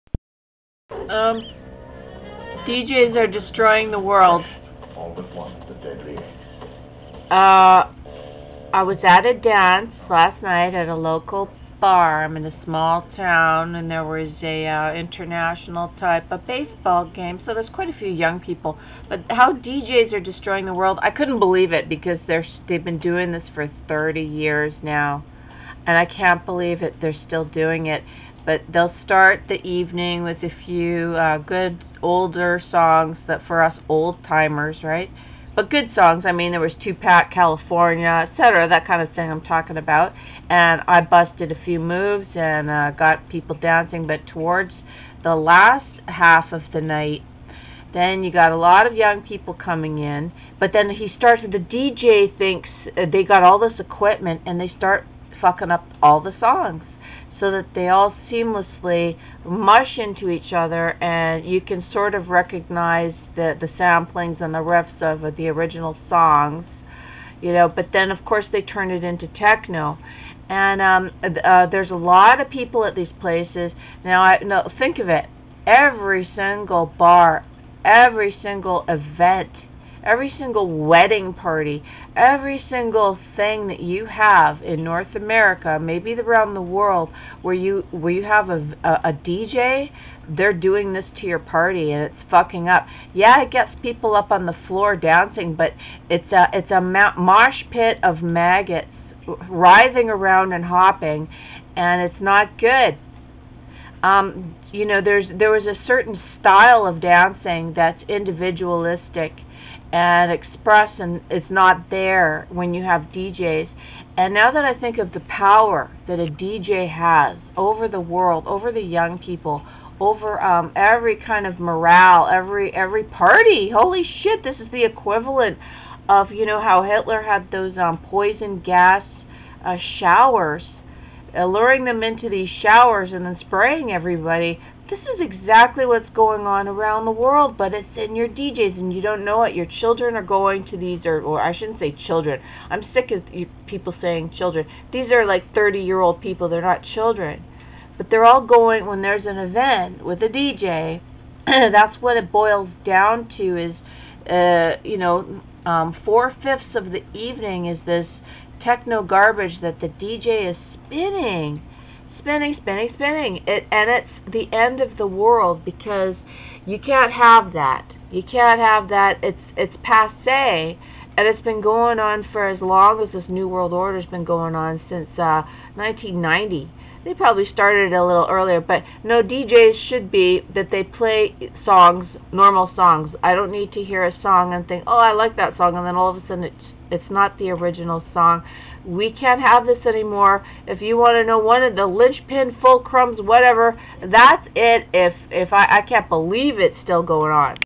deej@ys are destroying the world yes u heard it right
talk about destruction of music and dancing, morale, and all that goes with it. the proof is in the pudding 30 years of crap deejay mono control of ego driven weirdo's. it is NOT BENIGN